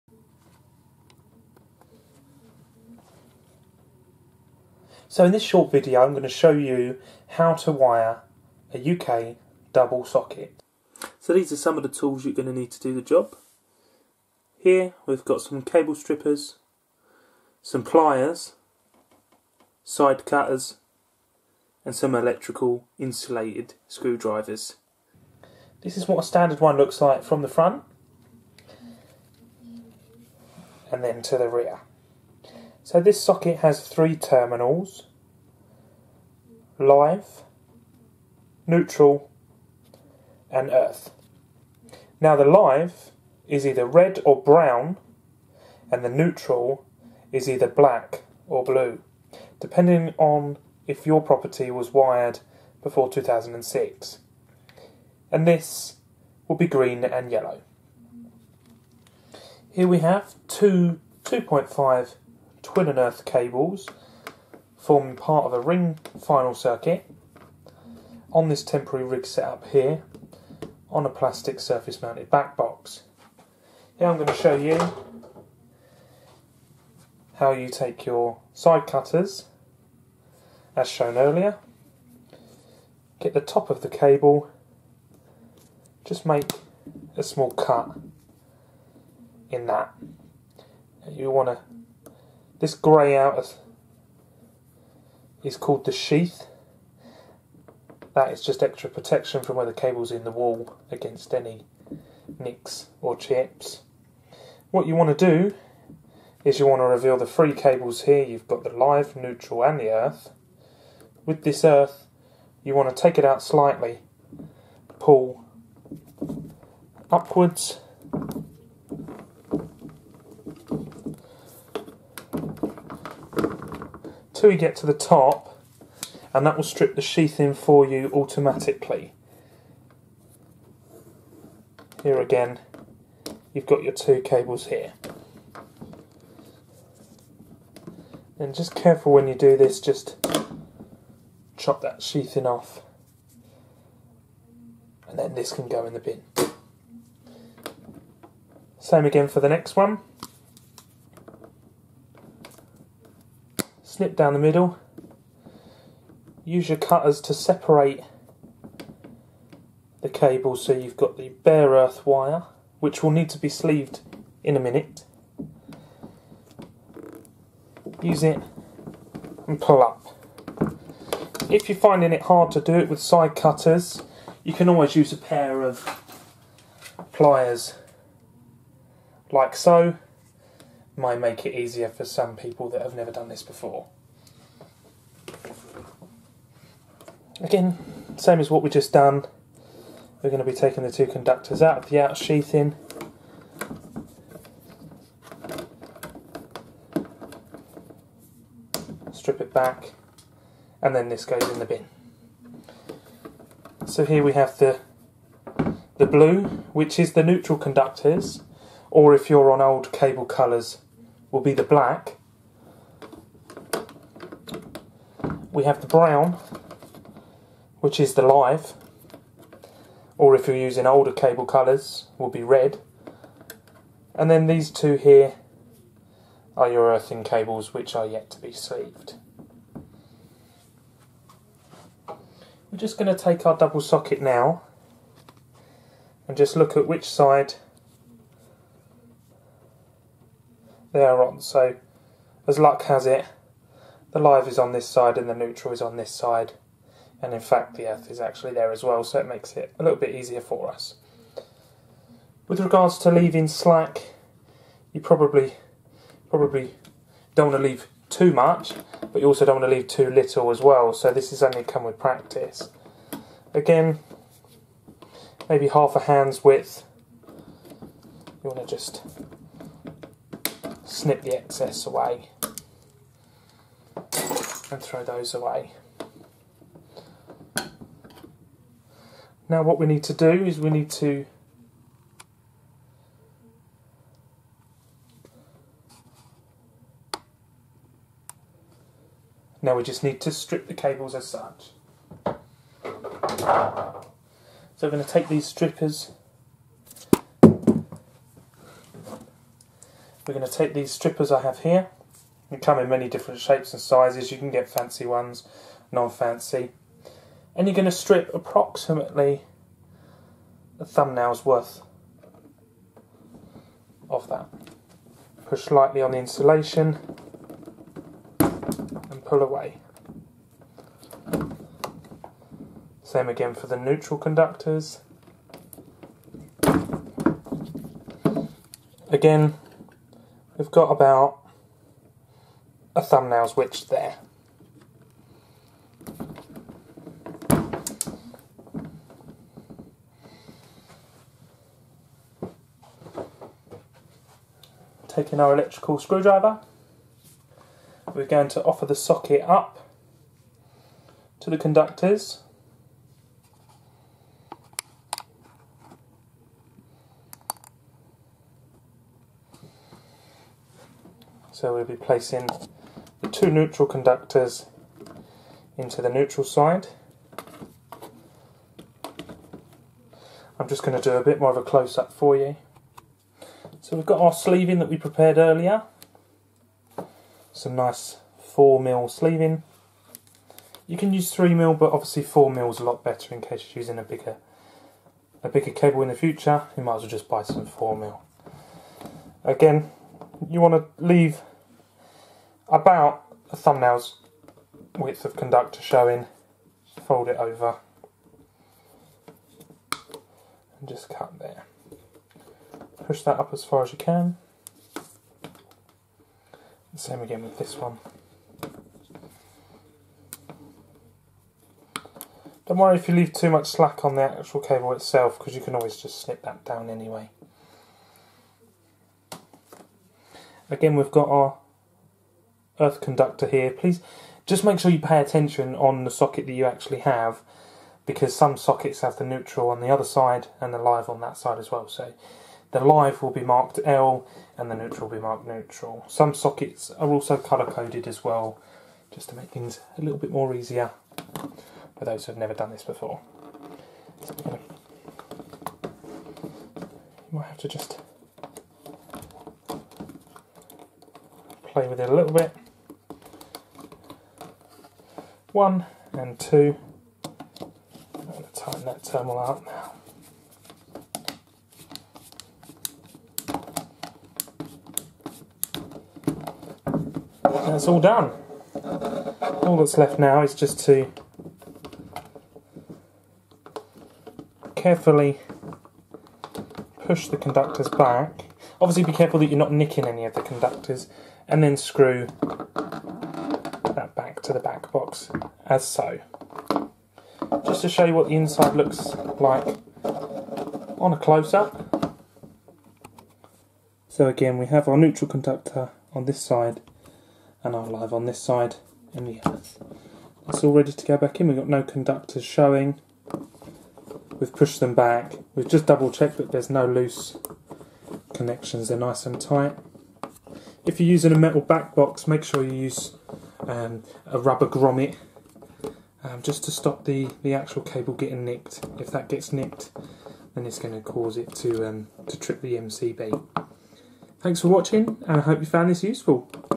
How To Wire A Double Socket
Step by step clear instruction video to follow when wiring a UK double socket.